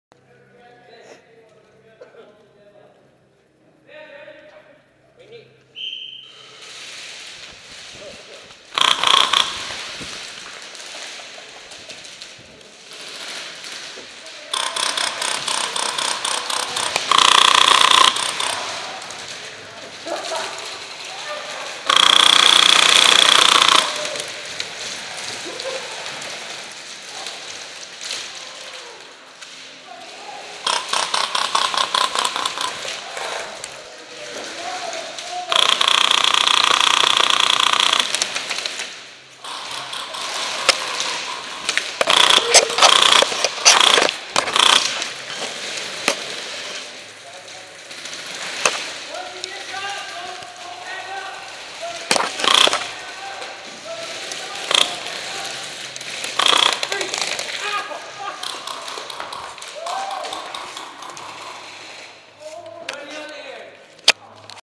mL Airsoft Terminator Game
Terminator Game at tac city. 3 guys went two rounds against everyone there.  I have to give them credit.

From Ben's Headcam.